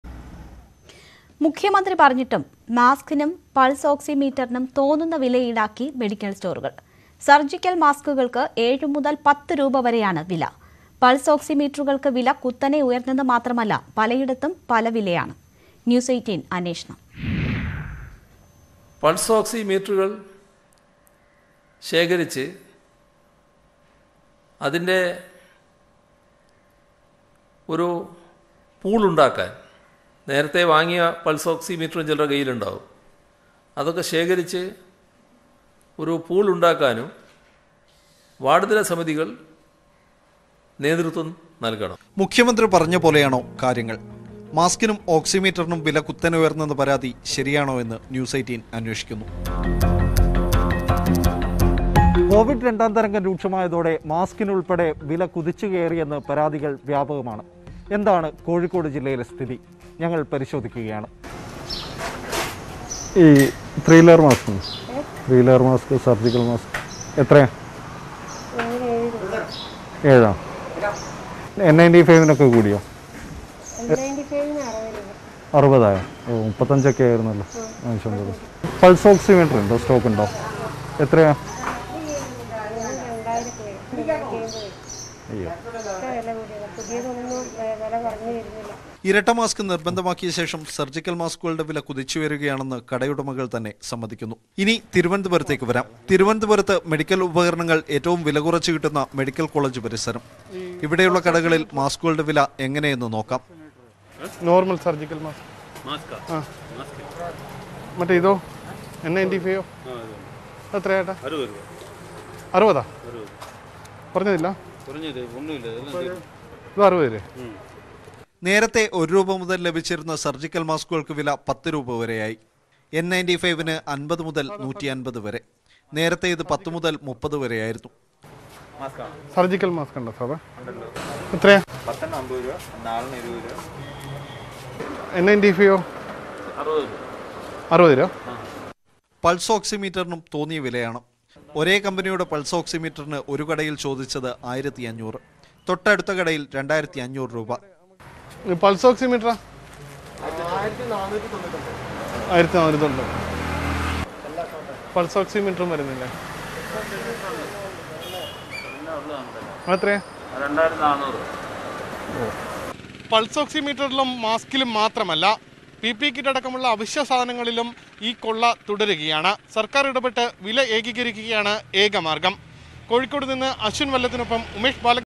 പൾസ് ഓക്സിമീറ്ററിന് വില കുത്തനെ ഉയരുന്നു; സർജ്ജിക്കൽ മാസ്കുകൾക്കും വില മുകളിലോട്ട്
മുഖ്യമന്ത്രി കൃത്യമായ നിർദേശം നൽകിയിട്ടും മാസ്കിനും പൾസ് ഓക്സിമീറ്ററിനും തോന്നുന്ന വില ഈടാക്കി മെഡിക്കൽ സ്റ്റോറുകൾ. ഡബിൾ മാസ്ക് നിർബന്ധമായതിനെ തുടർന്ന് സർജ്ജിക്കൽ മാസ്കുകൾക്കും വില കുത്തനെ ഉയരുകയാണ്.